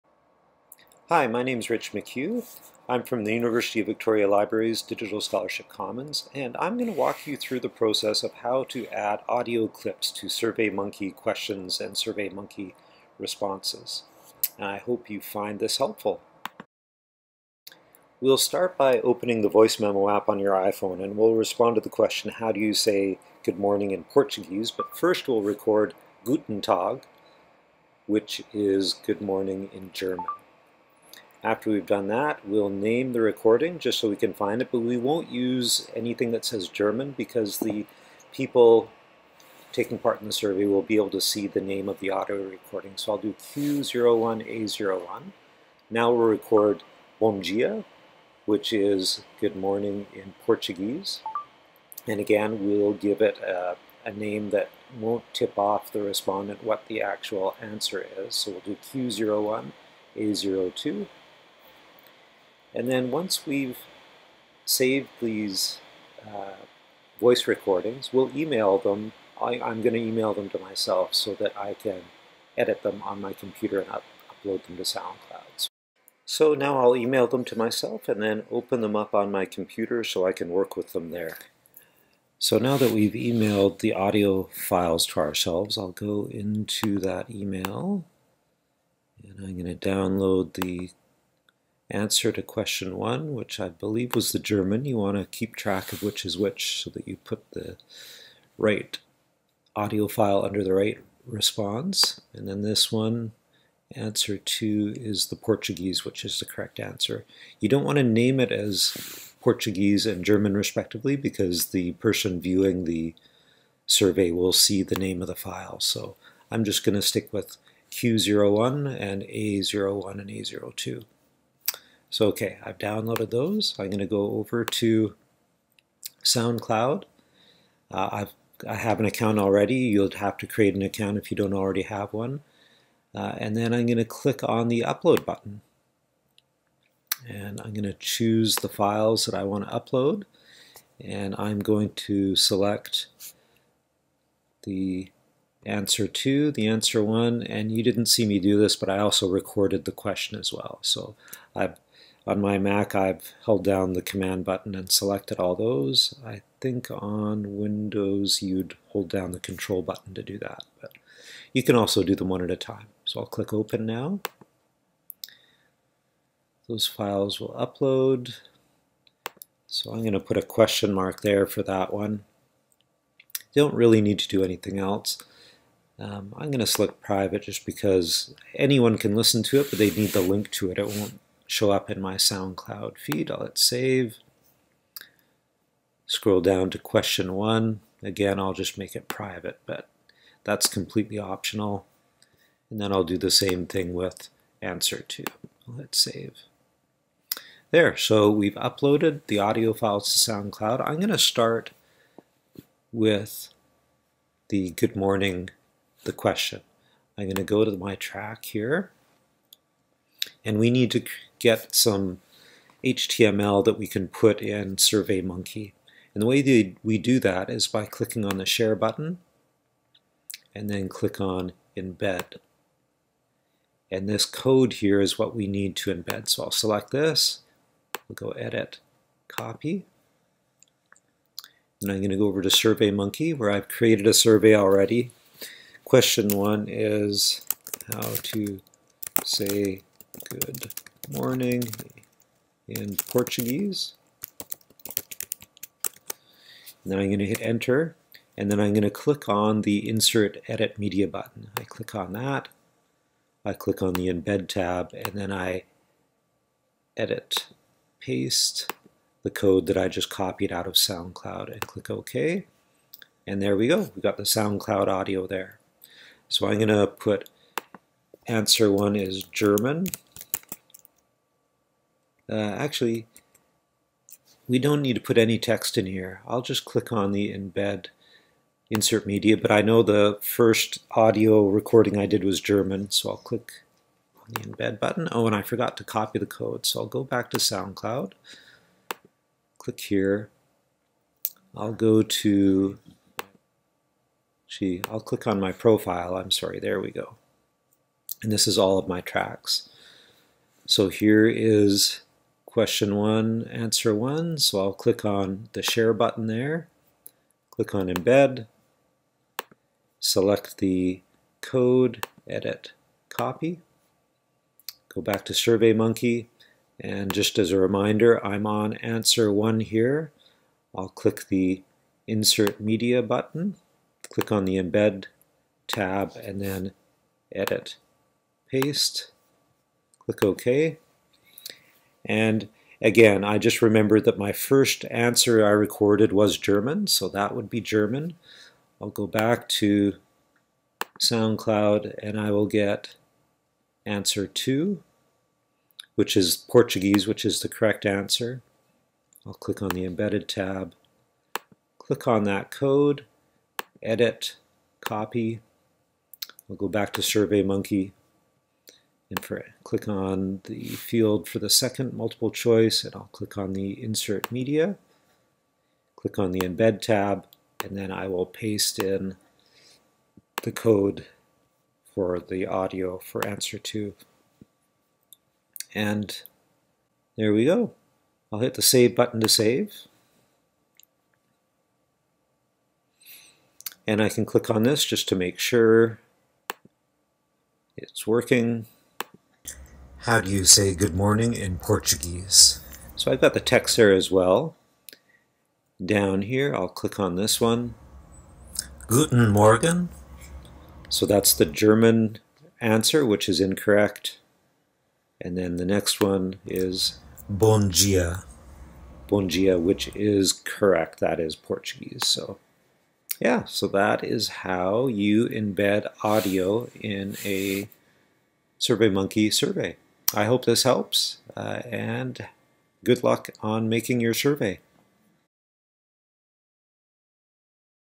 Adding Audio Clips to SurveyMonkey Questions
A short totorial to walk you through the process of adding audio clips to your SurveyMonkey question and/or answers.